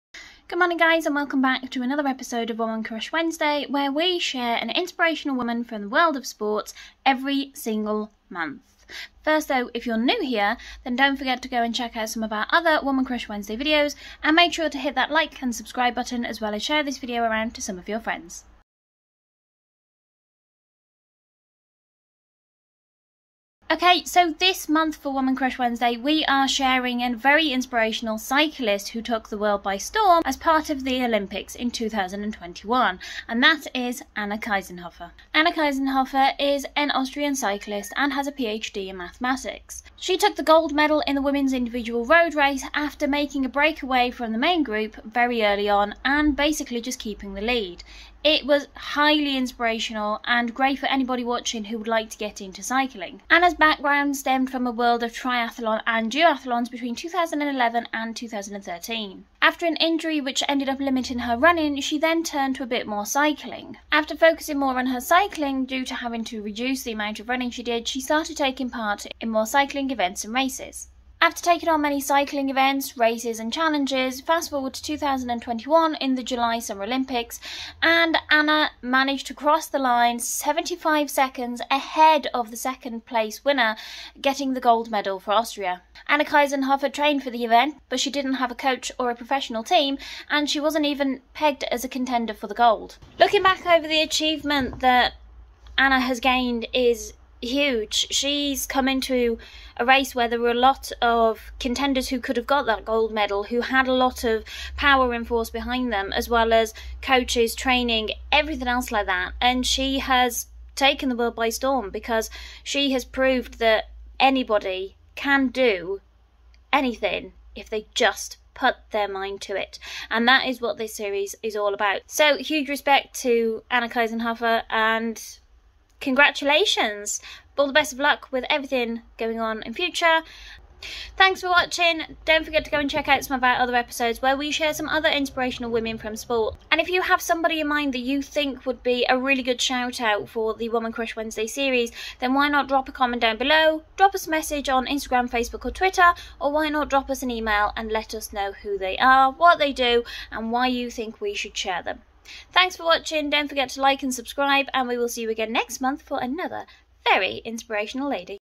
WomenCrushWednesday Anna Kiesenhofer
Back with another episode of women crush wednesday as we share the wonderful Olympian Anna Kiesenhofer
Designed for newcomers and veterans alike we aim to provide a healthy mix of humour and how-to to get you out and about, whether it be on your feet or on your bike